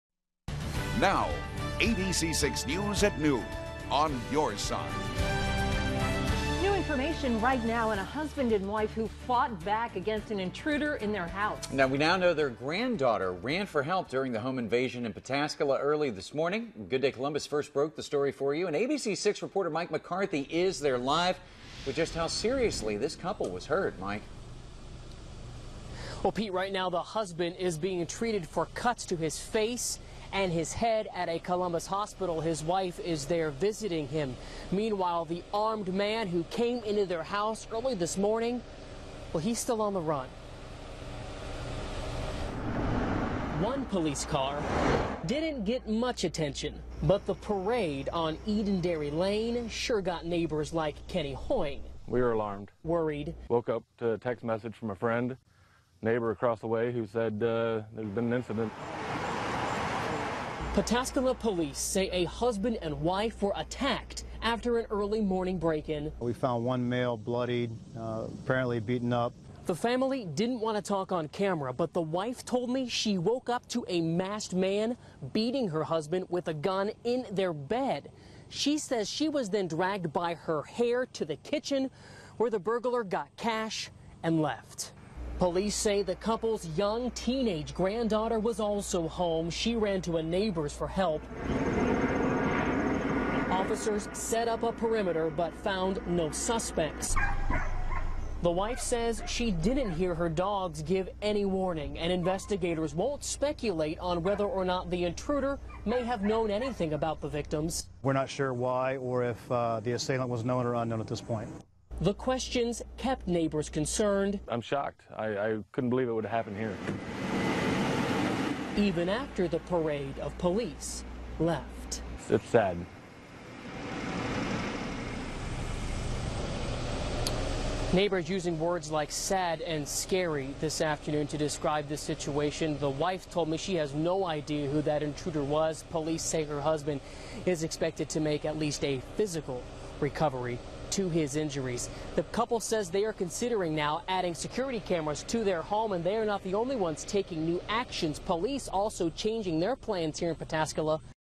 Pataskala Husband, Wife Fight Back Against Int
PATASKALA (Mike McCarthy/Sean Rowe/Chelsea Spears) -- It was a family effort